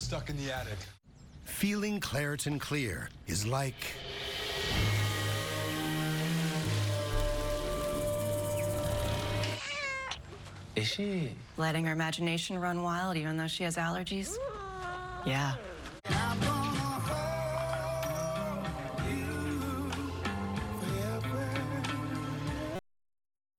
🌟 Children’s Claritin: Powerful Allergy Relief Without Sleepiness! 🌼tvcommercials
In this delightful Children’s Claritin commercial, watch as kids embrace the great outdoors without the burden of allergies. Our chewable tablets provide 24-hour non-drowsy allergy relief, so little ones can play, learn, and explore without feeling sleepy. Say goodbye to sneezing, runny noses, and itchy eyes—Children’s Claritin has got you covered! 🌿👧🌸